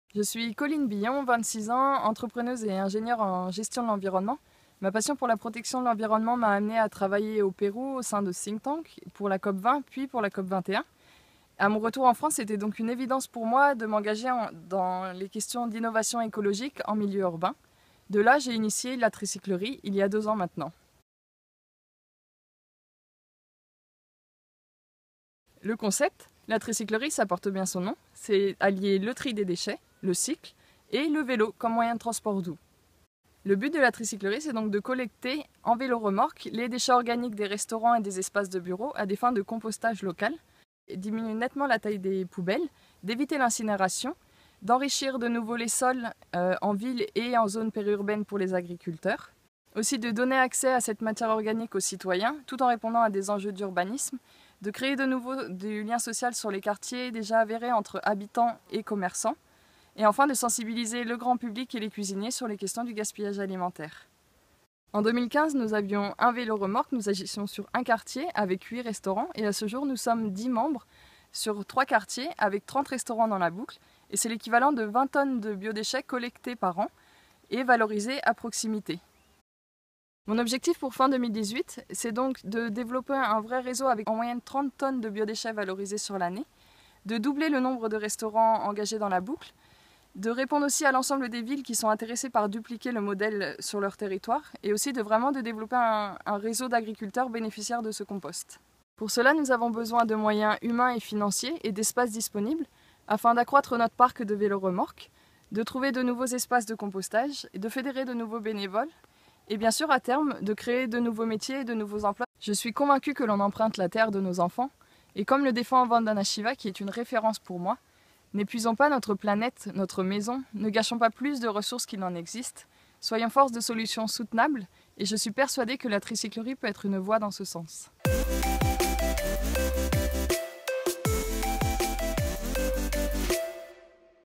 Young Champions of the Earth: Coline BIllon
Meet Coline Billon, a Young Champions of the Earth Europe Regional Finalist.